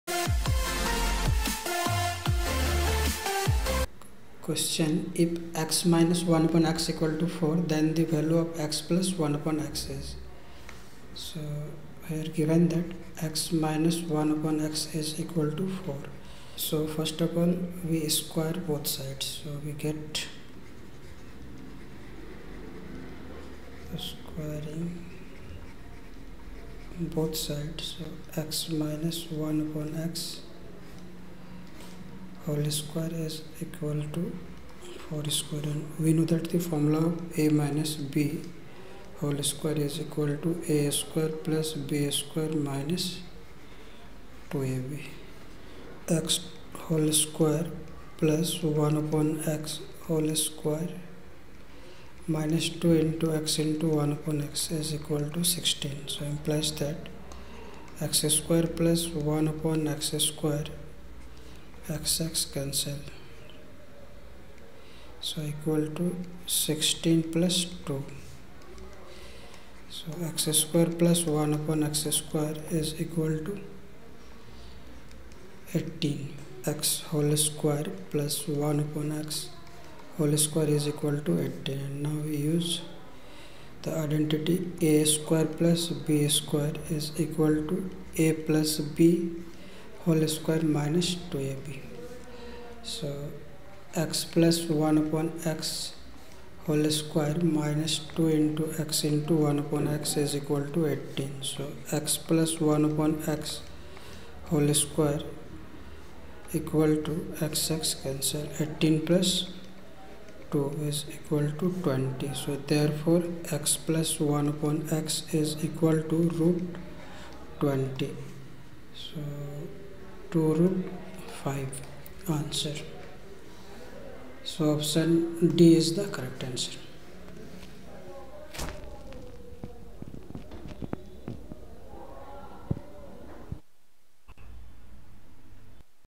Q111 | If x-1/x=4, then the value of x+1/x is | Algebra | Gravity Coaching Centre
Click on below Links for more Questions:-

Qu(111):- If x-1/x=4, then the value of x+1/x is:-